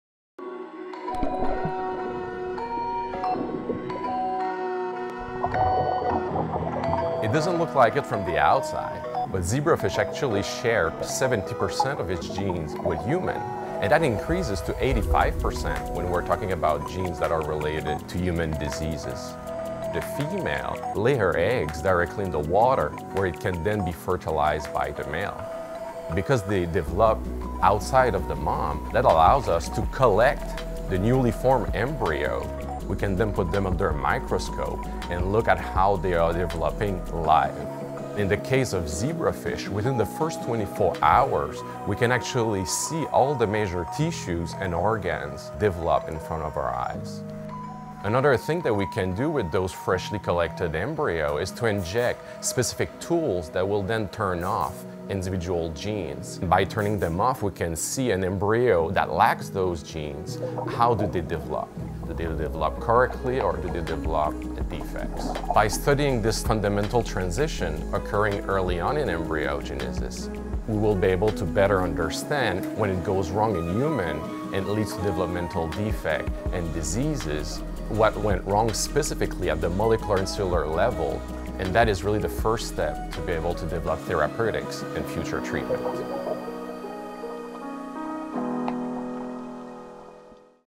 Science In Seconds: What Can We Learn from Zebrafish
Zebrafish have a similar genetic structure to humans which makes them ideal organisms to study. The UConn Health lab of Jean-Denis Beaudoin, associate professor of genetics and genome sciences, includes a fish facility that is home to approximately 20 thousand zebrafish. Beaudoin is studying the earliest stages of zebrafish development to better understand how defects and diseases develop in humans which could lead to future treatments.